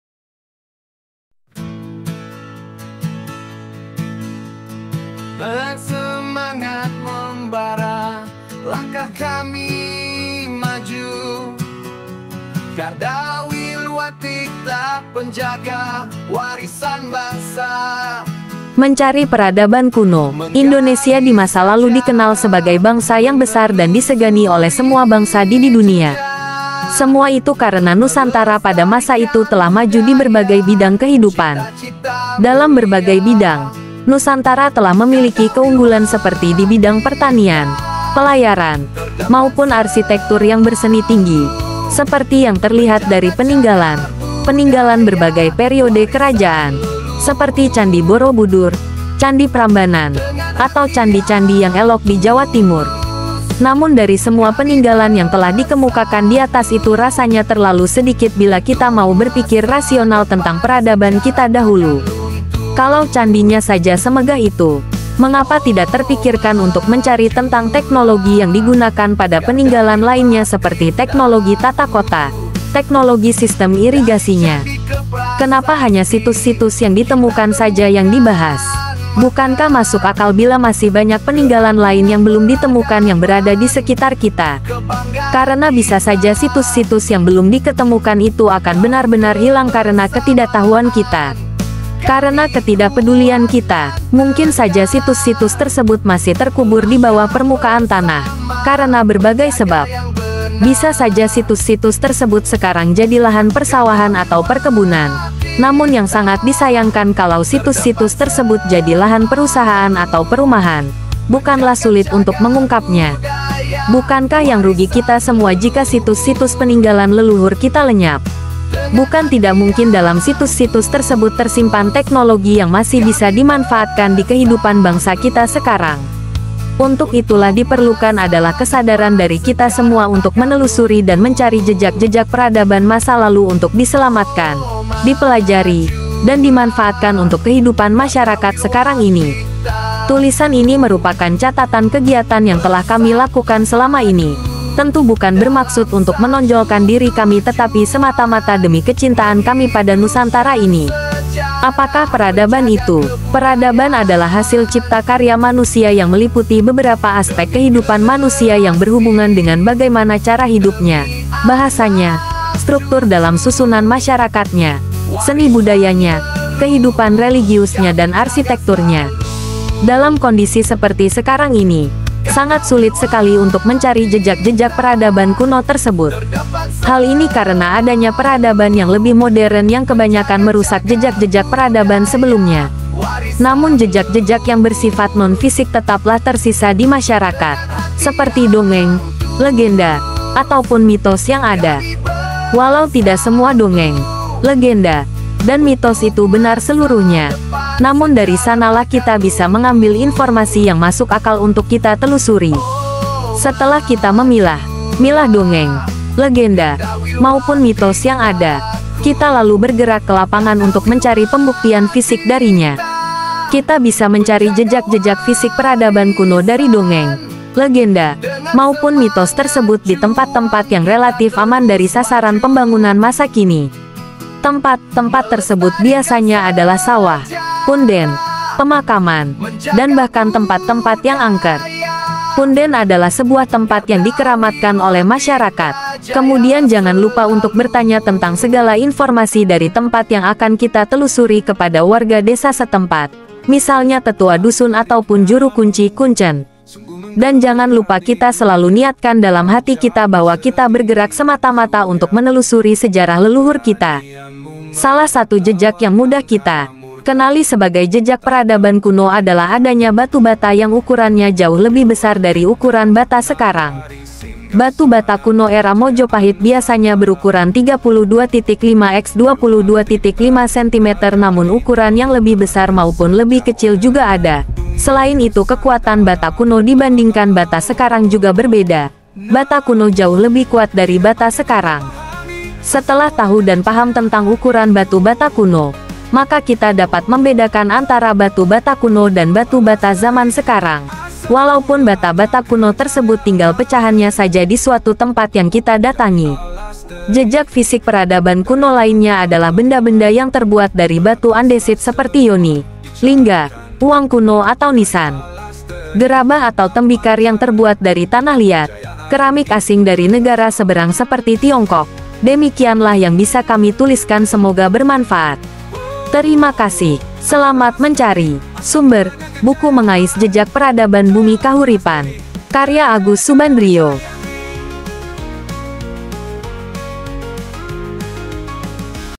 MENCARI PERADABAN KUNO
Indonesia di masa lalu dikenal sebagai bangsa yang besar dan disegani oleh semua bangsa di didunia . Semua itu karena Nusantara pada masa itu telah maju diberbagai bidang kehidupan.
           Dalam berbagai bidang , Nusantara telah memiliki keunggulan seperti di bidang pertanian , pelayaran , maupun arsitektur yang seni tinggi , seperti yang terlihat dari peninggalan - peninggalan berbagai periode kerajaan , seperti Candi Borobudur , Candi Prambanan , atau Candi- Candi yang elok di Jawa Timur .
        Namun dari semua peninggalan yang  telah dikemukakan diatas itu rasanya terlalu sedikit bila kita mau berpikir rasional tentang peradaban kita dahulu . Kalau candi nya saja semegah itu , mengapa tidak terpikirkan  untuk mencari tentang teknologi yang digunakan pada peninggalan lainnya seperti teknologi tata kota , teknologi sistem irigasi nya? Kenapa hanya situs- situs yang ditemukan saja yang di bahas? Bukankah masuk akal bila masih banyak peninggalan lain yang belum ditemukan yang berada disekitar kita?
          Karena bisa saja situs-situs yang belum diketemukan itu akan benar-benar hilang karena ketidaktahuan kita? Karena ketidakpedulian kita? Mungkin saja situs-situs tersebut masih terkubur dibawah permukaan tanah , karena berbagai sebab . Bisa saja situs-situs tersebut sekarang jadi lahan persawahan atau perkebunan , namun yang sangat disayangkan kalau situs-situs tersebut jadi lahan perusahaan atau perumahan , bukanlah sulit untuk mengungkap nya?
           Bukankah yang rugi kita semua jika situs- situs peninggalan leluhur kita lenyap ? Bukan tidak mungkin dalam situs-situs tersebut tersimpan teknologi yang masih bisa dimanfaatkan di kehidupan bangsa kita sekarang ?
         Untuk itulah diperluk Y6an kesadaran dari kita semua untuk menelusuri dan mencari jejak-jejak peradaban masa lalu untuk diselamatkan , dipelajari , dan dimanfaatkan untuk kehidupan masyarakat sekarang ini . Tulisan ini merupakan catatan kegiatan yang telah kami lakukan selama ini , tentu bukan bermaksud untuk menonjolkan diri kami tetapi semata-mata demi kecintaan kami pada Nusantara ini.

        Apakah peradaban itu ? Peradaban adalah hasil cipta karya manusia yang meliputi beberapa aspek kehidupan manusia yang berhubungan dengan bagaimana cara hidup nya , bahasa nya , struktur dalam susunan masyarakat nya , seni budaya nya , kehidupan religius nya dan arsitektur nya.
       Dalam kondisi seperti sekarang ini , sangat sulit sekali untuk mencari jejak- jejak peradaban kuno tersebut . Hal ini karena adanya peradaban yang lebih modern yang kebanyakan merusak jejak-jejak peradaban sebelum nya .
        Namun jejak-jejak yang bersifat non fisik tetap lah tersisa di masyarakat , seperti dongeng , legenda , ataupun mitos yang ada . Walau tidak semua dongeng , legenda , dan mitos itu benar seluruhnya , namun dari sana lah kita bisa mengambil informasi yang masuk akal untuk kita telusuri . Setelah kita memilah - Milah dongeng , legenda , maupun mitos yang ada , kita lalu bergerak ke lapangan untuk mencari pembuktian fisik darinya .
      Kita bisa mencari jejak-jejak fisik peradaban kuno dari dongeng , legenda , maupun mitos tersebut di tempat- tempat yang relatif aman dari sasaran pembangunan masa kini . Tempat - tempat tersebut biasanya adalah sawah , punden , pemakaman , dan bahkan tempat- tempat yang angker . Punden adalah sebuah tempat yang dikeramatkan oleh masyarakat .
       Kemudian jangan lupa untuk bertanya tentang segala informasi dari tempat yang akan kita telusuri kepada warga desa setempat , misalnya Tetua Dusun ataupun Juru Kunci ( Kuncen) . Dan jangan lupa kita selalu niatkan dalam hati kita bahwa kita bergerak semata-mata untuk menelusuri sejarah leluhur kita .
      Salah satu jejak yang mudah kita kenali sebagai jejak peradaban kuno adalah adanya batu bata yang ukurannya jauh lebih besar dari ukuran bata sekarang . Batu bata kuno era Mojopahit biasanya berukuran 32.5 x 22.5 sentimeter namun ukuran yang lebih besar maupun lebih kecil juga ada , selain itu kekuatan bata kuno dibandingkan bata sekarang juga berbeda , bata kuno jauh lebih kuat dari bata sekarang .
      Setelah tahu dan paham tentang ukuran batu bata kuno , maka kita dapat membedakan antara batu bata kuno dan batu bata zaman sekarang , walaupun bata- bata kuno tersebut tinggal pecahannya saja di suatu tempat yang kita datangi .
Jejak fisik peradaban kuno lainnya adalah benda-benda yang terbuat dari batu Andesit seperti Yoni , lingga , uang kuno atau nisan . Gerabah atau tembikar yang terbuat dari tanah liat , keramik Asing dari negara seberang seperti Tiongkok , demikianlah yang bisa kami tuliskan semoga bermanfaat .

Terimakasih ...

Selamat mencari .

Sumber : Buku Mengais Jejak Peradaban Bumi Kahuripan .